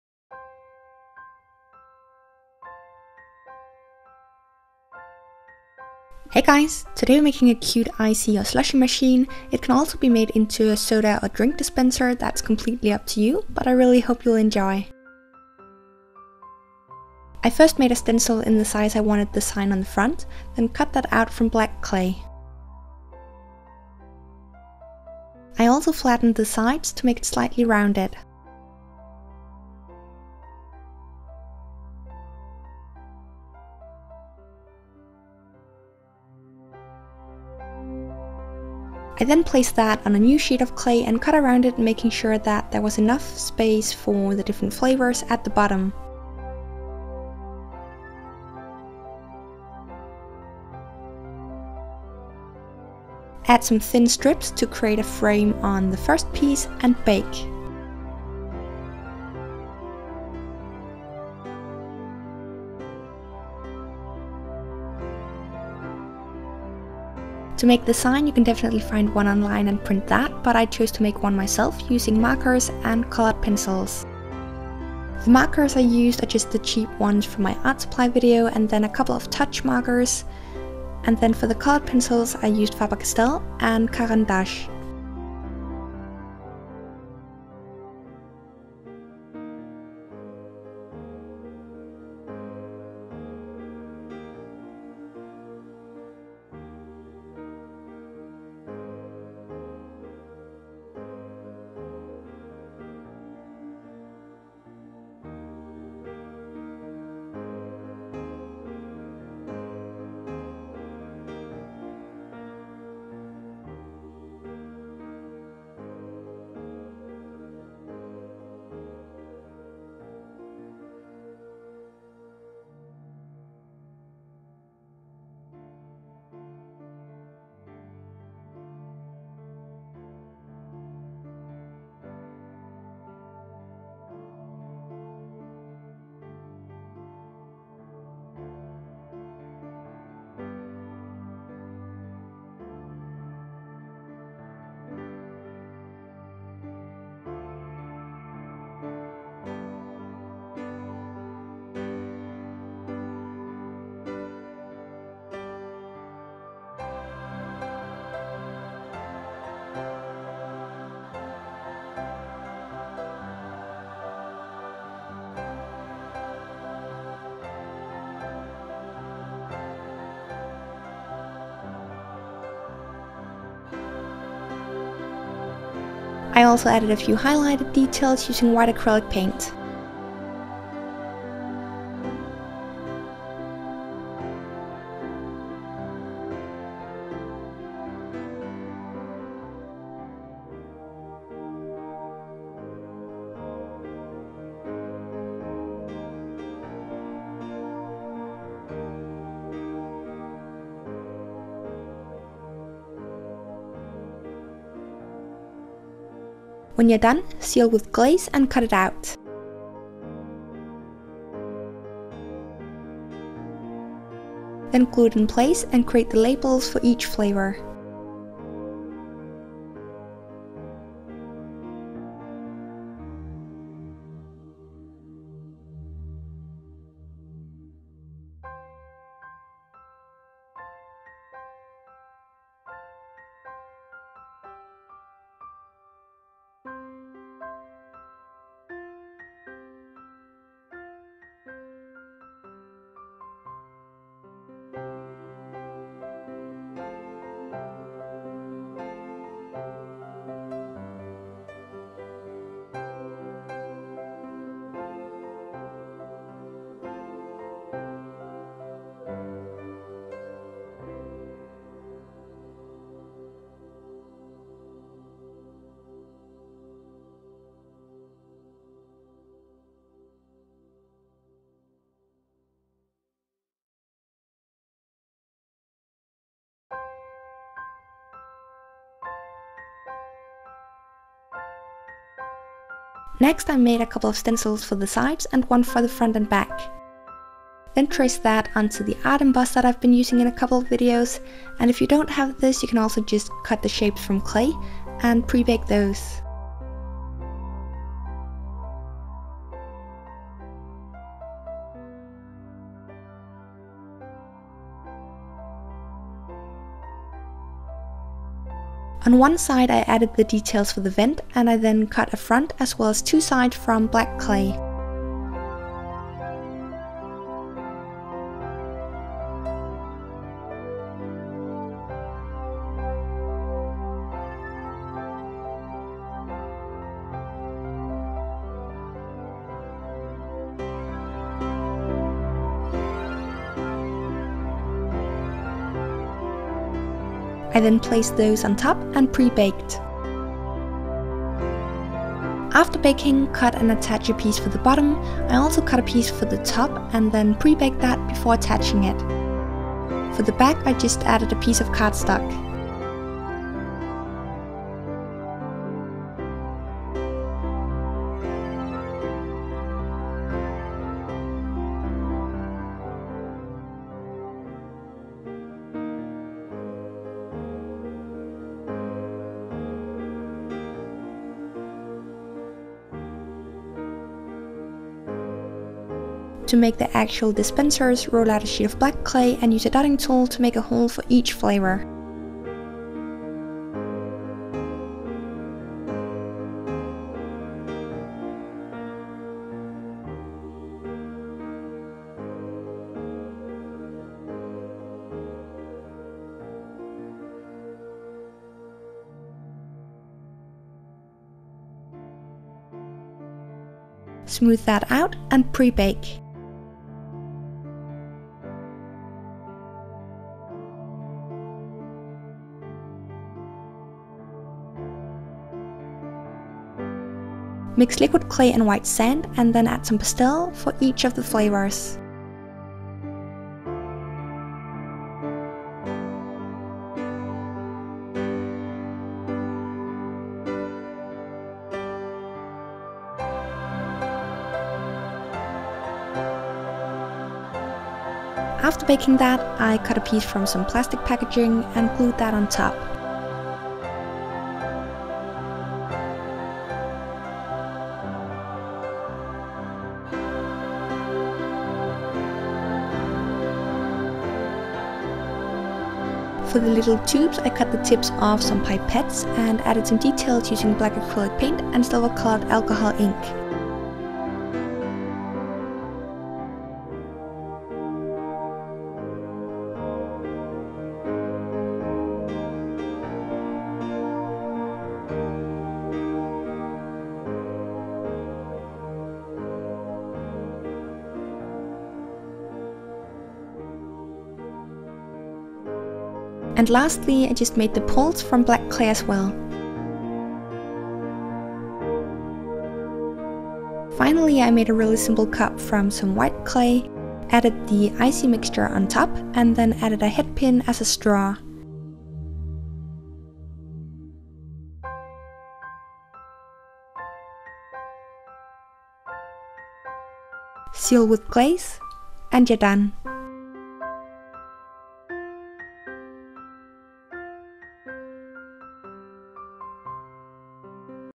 Miniature Slushie / Icee Machine - Polymer Clay Tutorial
Hey Guys! Today we're making a slush ice or icee machine for Dolls and dollhouses from polymer clay :) - you can also make it into a soda/drink dispenser, if that's more your style ^^